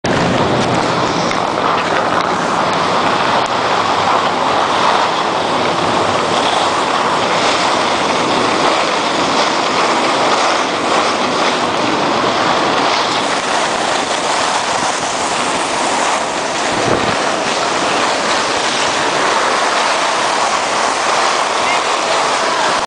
Daz Mac Skiing - Knee Cam
Daz Mac skiing at Tamworth Snowdome October 2008